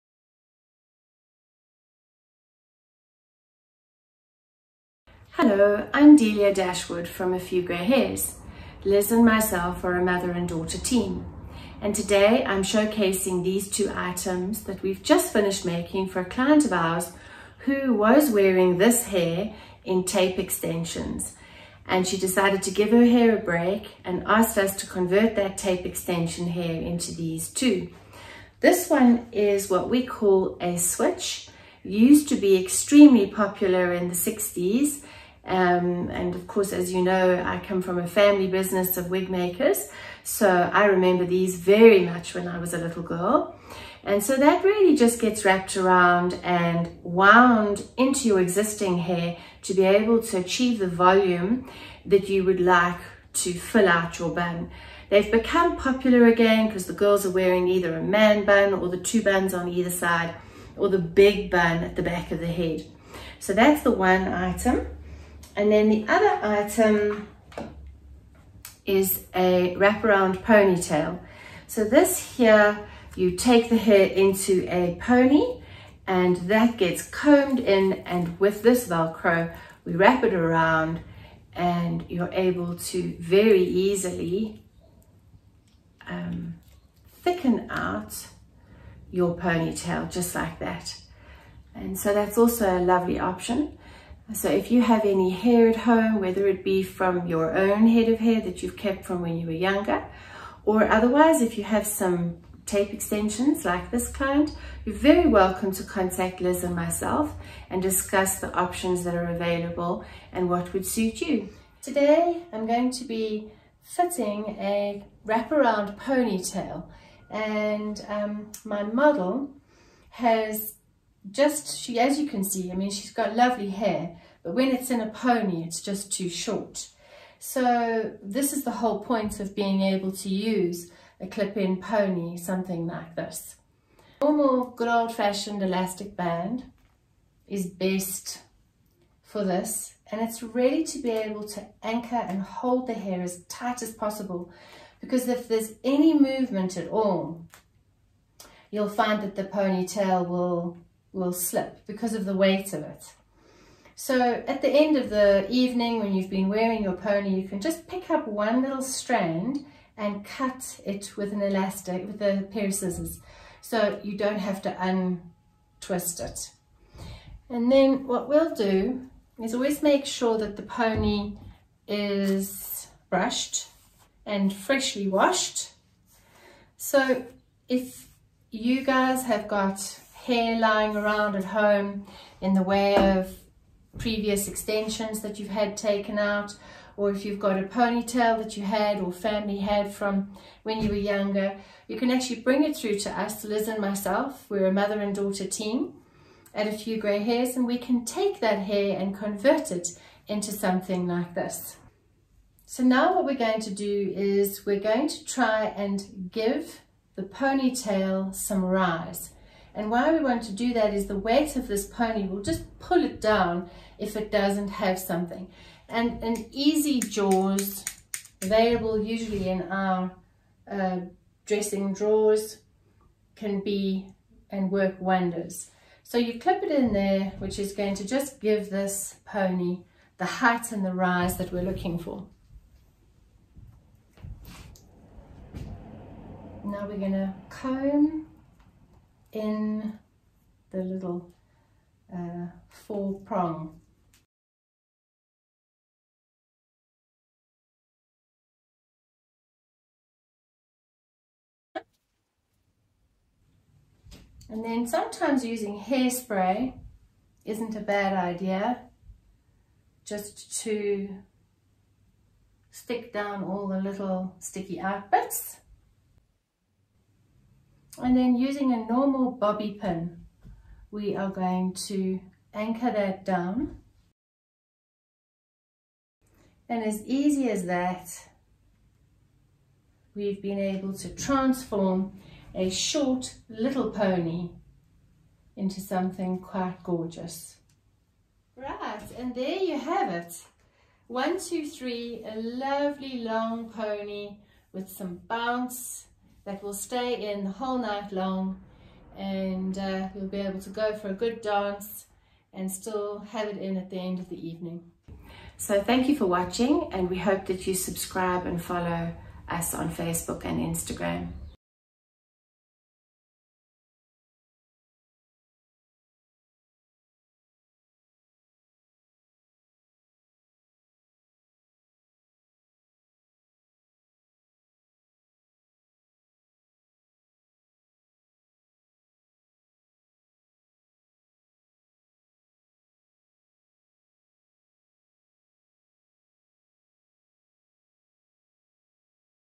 Delia from 'A Few Grey Hairs' demonstrates how to attach a wrap-around ponytail quickly and easily.
Delia demonstrates how easy it is to attach an add-on ponytail to extend the length of your hair.  Delia at 'A Few Grey Hairs' offer the service of using your own hair or hair from extensions and converting this hair into the many different options of hair replacements available.